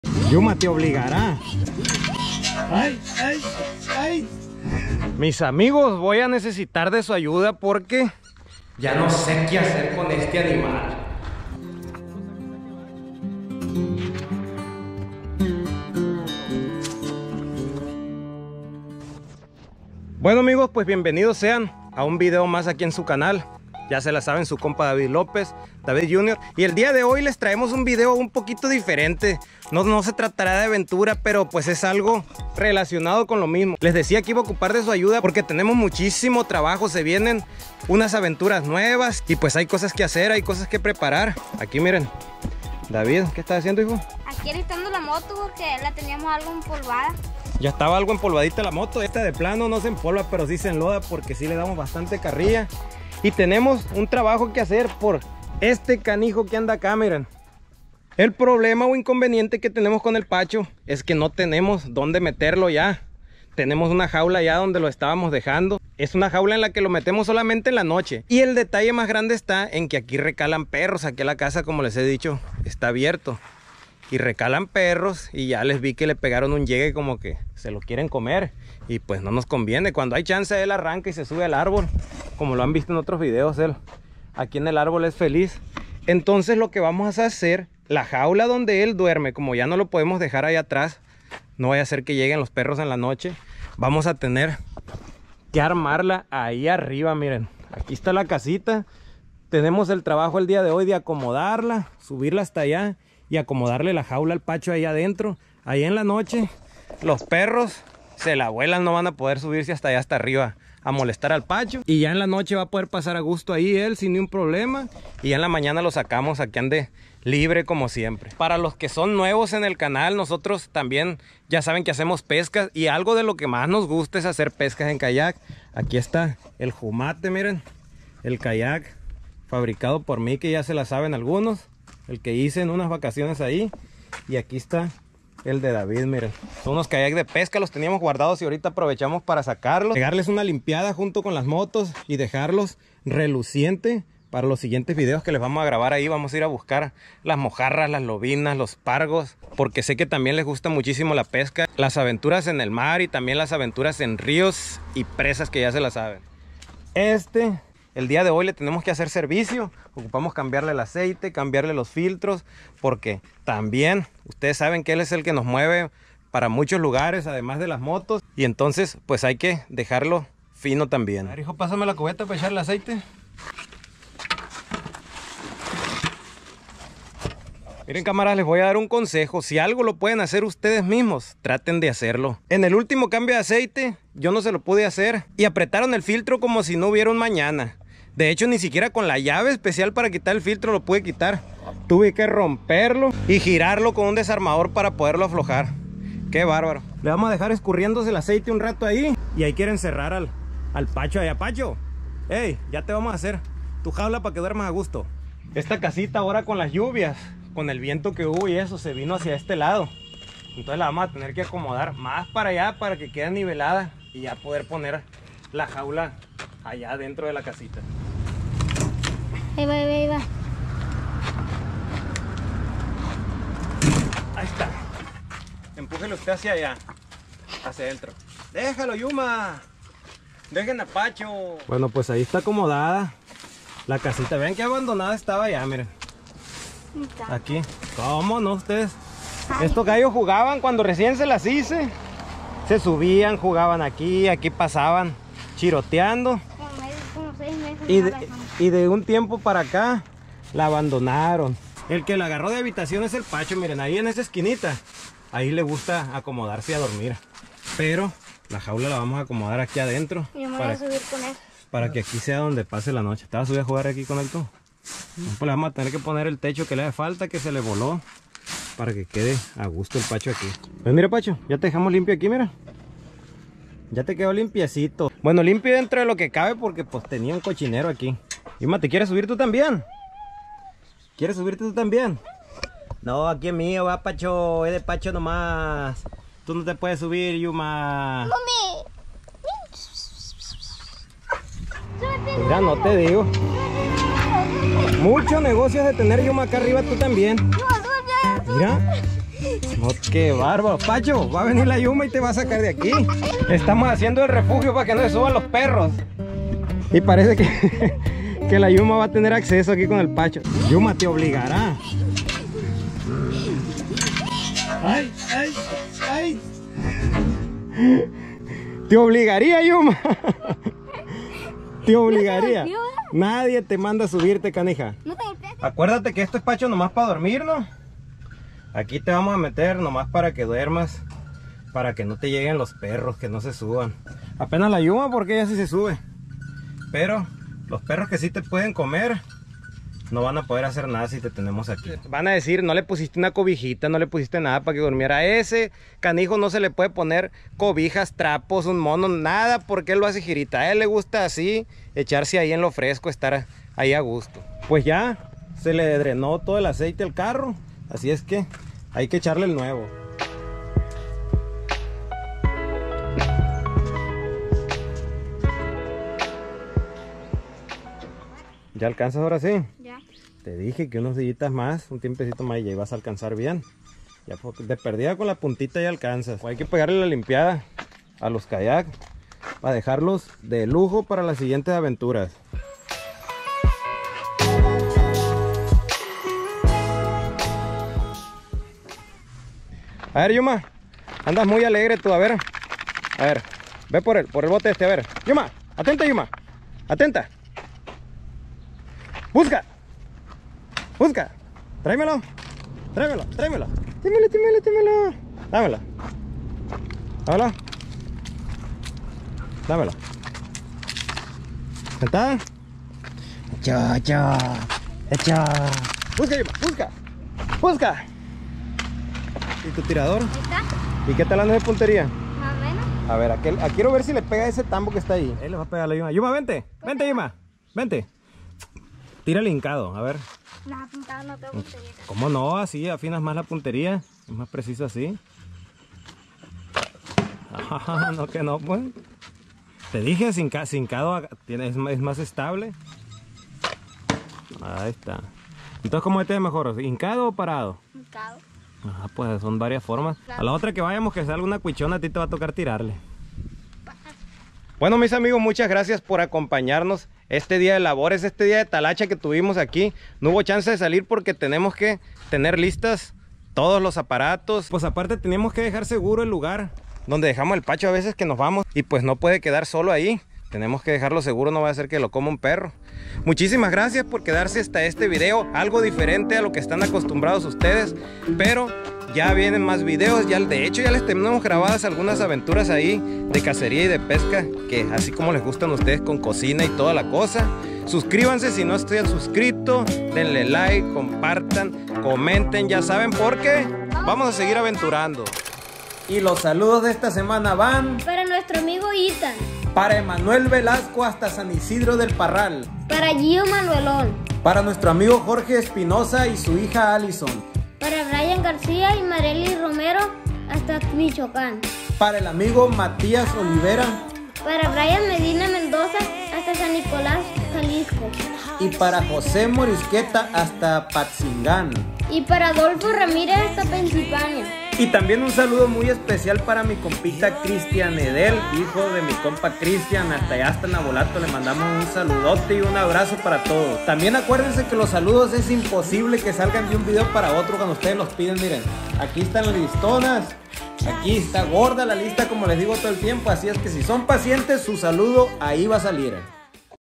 No sabemos que hacer con PACHO 🦝 🤔// Preparado LOS EQUIPOS para LAS SIGUIENTES AVENTURAS‼️🔥👌
En este video les traemos un dia de preparación de los equipos que utilizamos para las aventuras de pesca y caería, ademas tenemos la tarea de hacer un refugio para nuestra mascota PACHO el mapache ya que algunos perros lo han estado molestando.
dejamos el bass jeep, las motos y los kayaks listos ya que en próximos videos les traeremos mas caza y cocina de palomas y otras presas, asi como tambien pesca y cocina de mojarra, bagres, lobinas (bass) y otros peces de agua salada.

TIKTOK 👉🏻 .tiktok.com/@pescayaventuracondavid